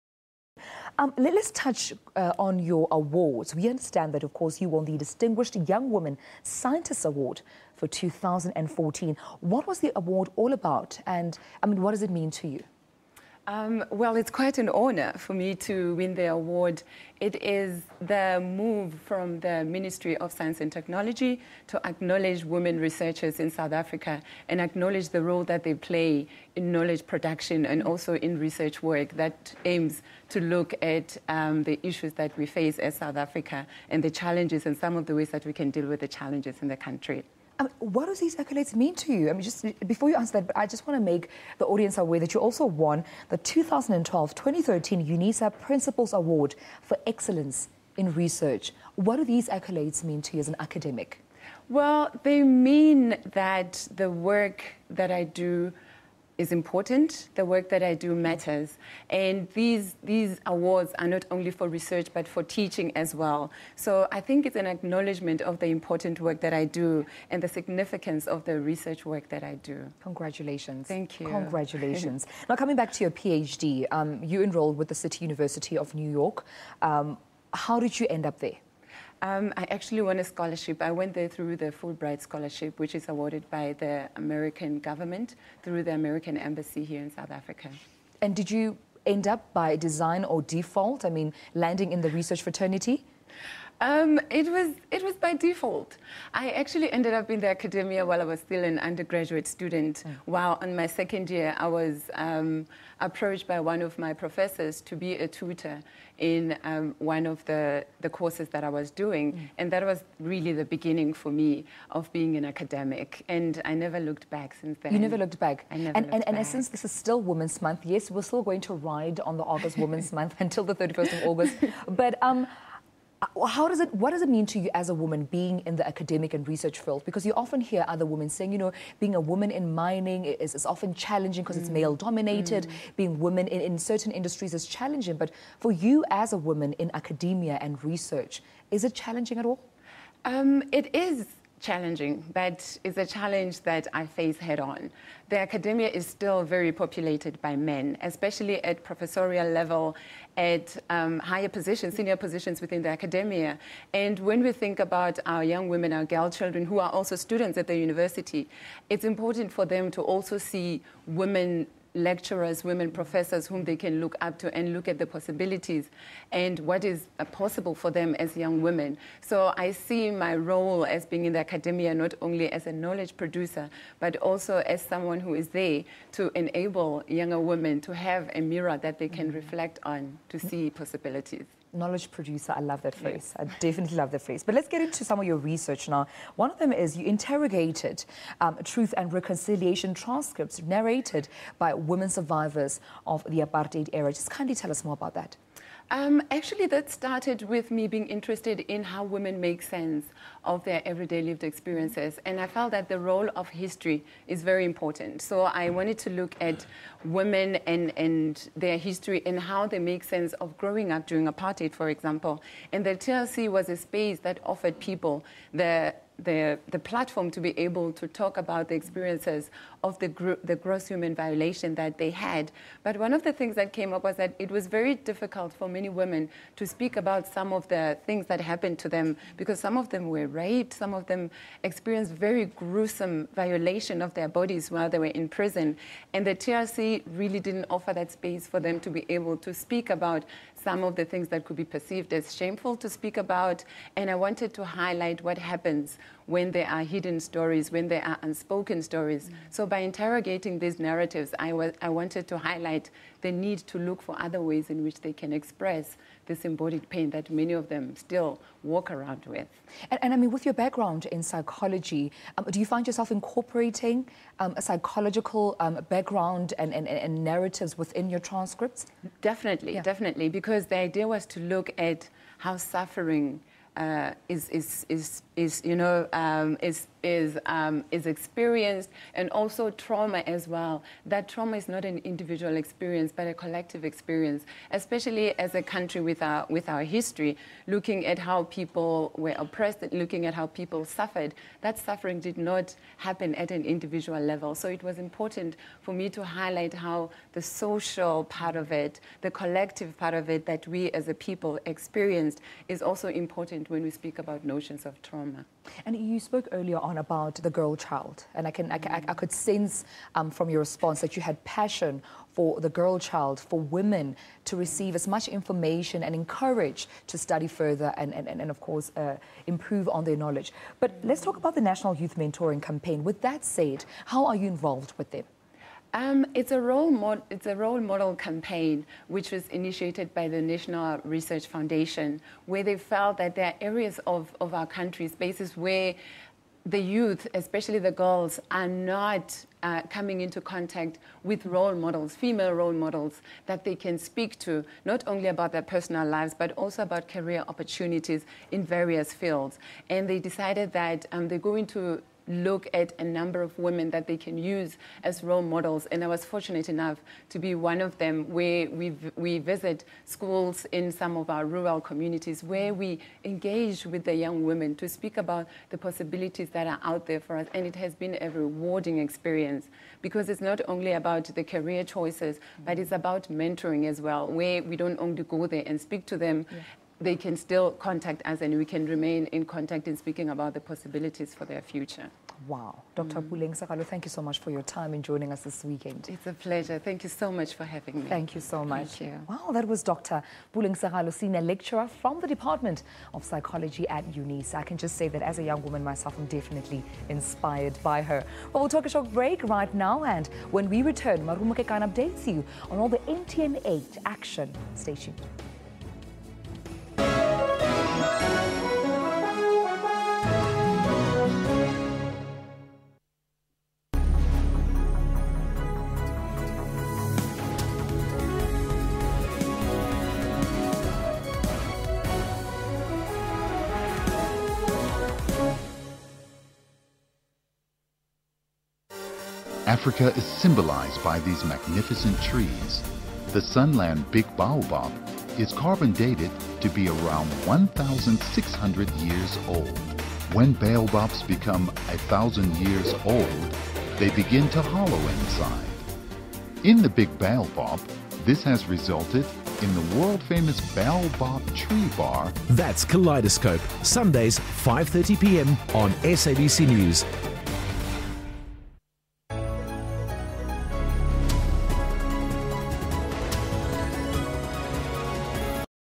Puleng Segalo won Distinguished Young Woman Scientists Award 2014
Dr Puleng Segalo has won the Distinguished Young Woman Scientists Award 2014 in the category of humanities and social sciences. She received her PhD in Social and Personality Psychology with double distinctions from the Graduate Centre, City University of New York in 2013. She is currently a Senior Lecturer in the Department of Psychology at the University of South Africa, UNISA. Dr Segalo is here with us in our studios, to give more information about her accolades.